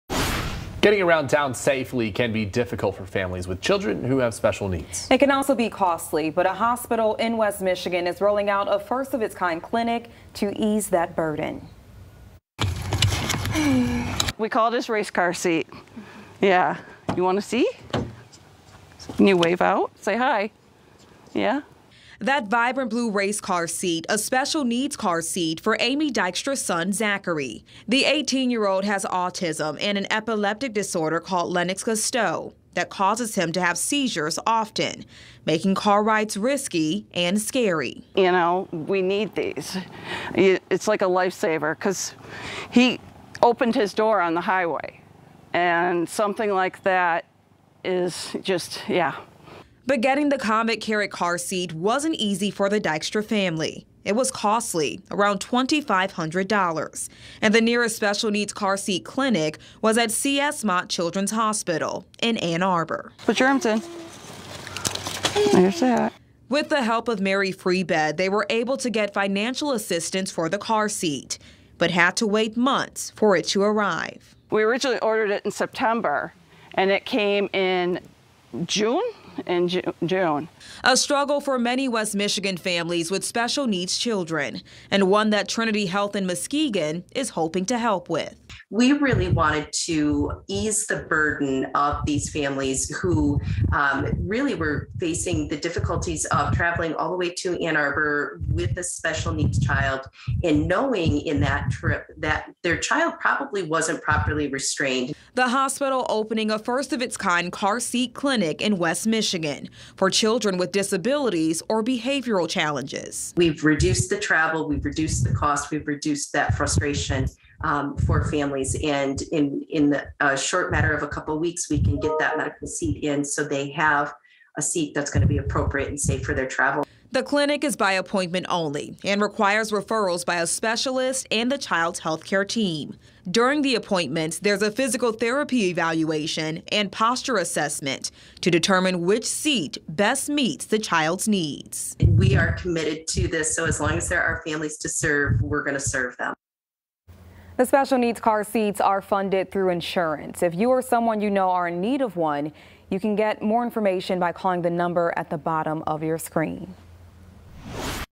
Special needs car seat clinic
Special needs car seat clinic. Trinity Health Muskegon providing assistance to families.